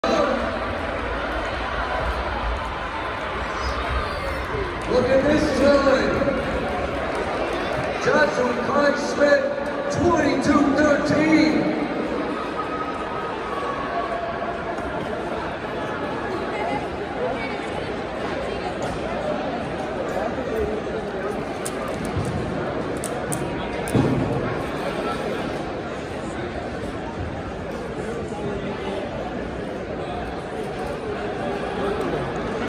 6.42m Long Jump at Adidas Nationals 2023
My 21ft .75inch/6.42M long jump at Adidas Nationals 2023, National Elite, Heat 4. Placed 14th overall.

It's not my PR (6.48m), but it's getting there.